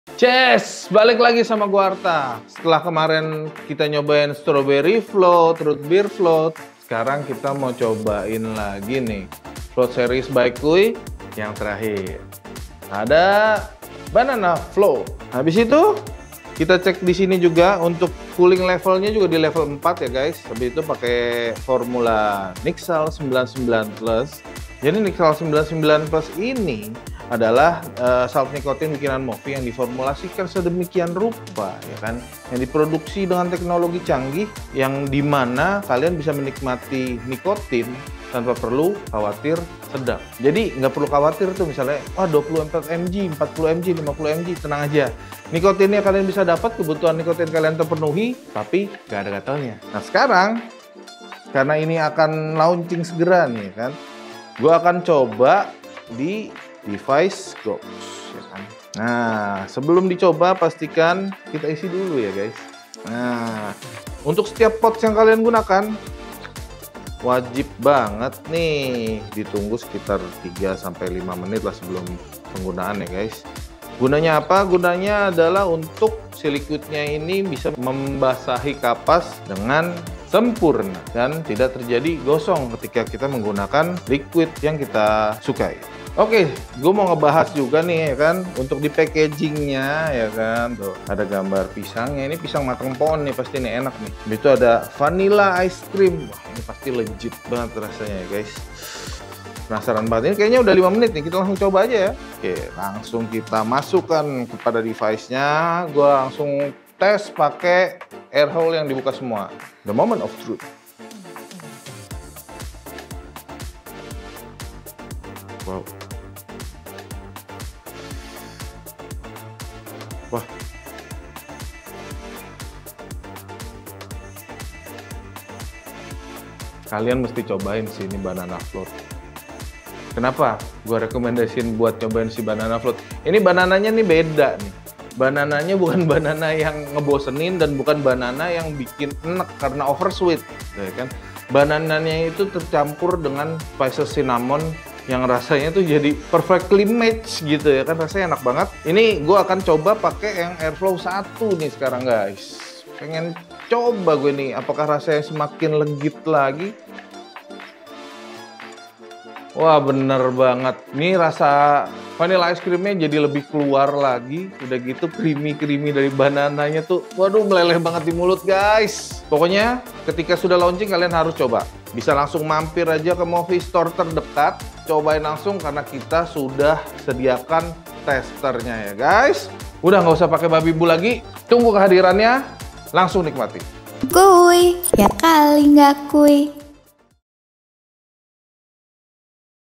Banana Float Review
Kali ini Bro Artha akan bahas liquid terbaru dari KUY Float series yaitu
KUY Banana Float
kalo kemarin udah ada deskripsi dari temen-temen yang dapet Early Adopter,
nah sekarang kita kasih penjelasan lewat video biar kalian bisa bayangin gimana rasa dan experiencenya,
Langsung simak videonya aja yuk.
btw KUY Banana Float ini udah tersedia di seluruh MOVI STORE, dan di Vapestore kesayangan kalian lhoo..